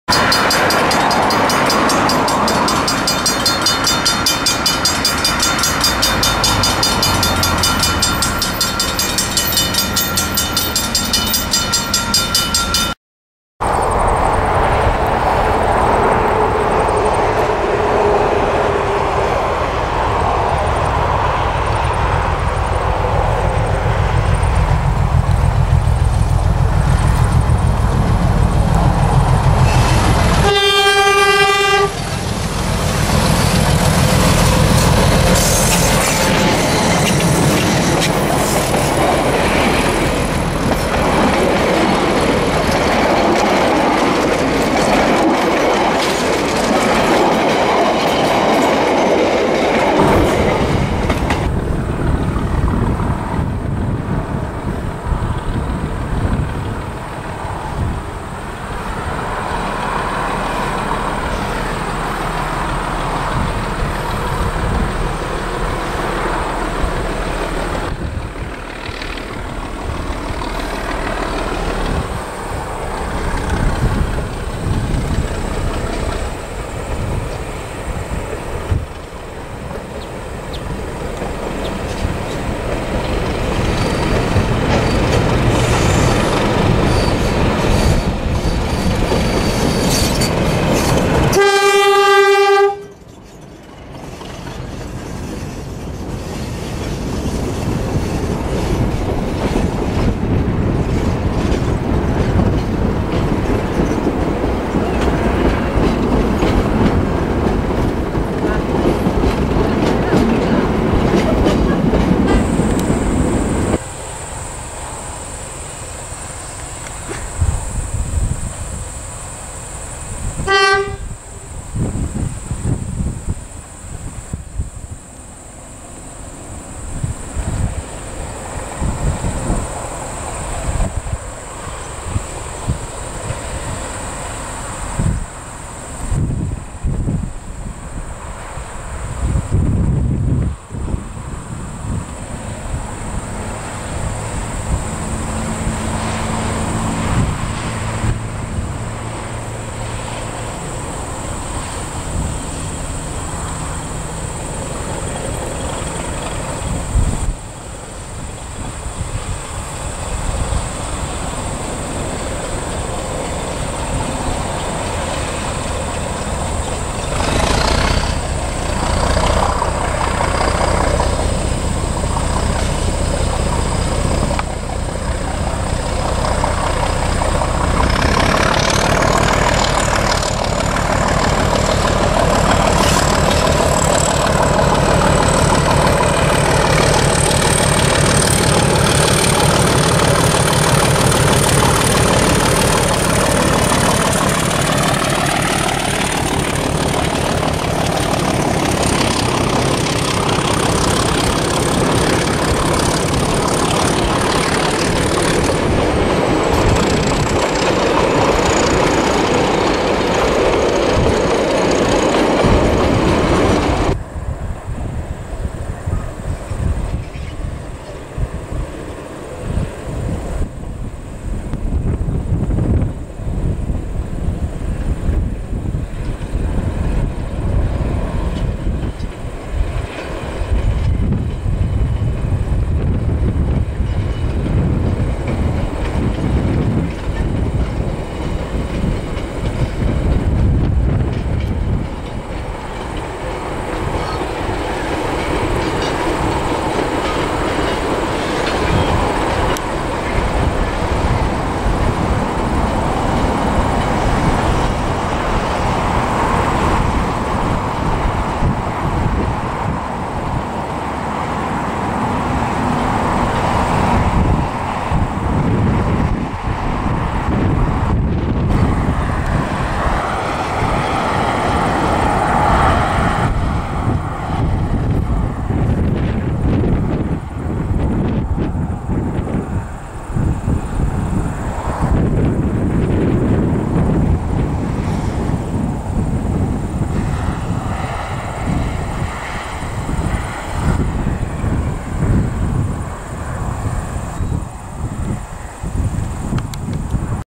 DG770 through the Weka Pass
Druing Easter Monday 2013, The Weka Pass Railway ran their DG770 locomotive through thier scenic line from Glenmark to Waikari.